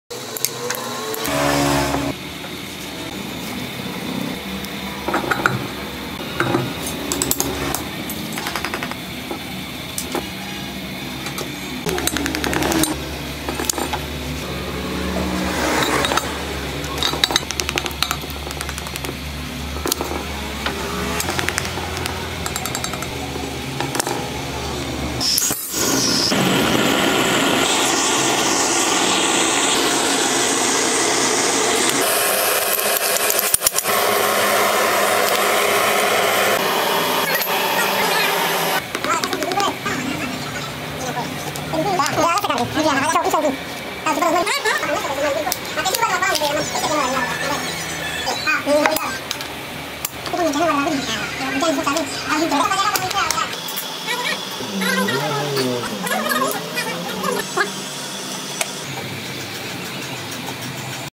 Amazing Restoration Technique of an Old Lead Acid Battery
In this video I show you step by step how I have successfully restored many Lead Acid batteries and the proper solutions mixture that has worked for me on 95% of all batteries I've tried it on.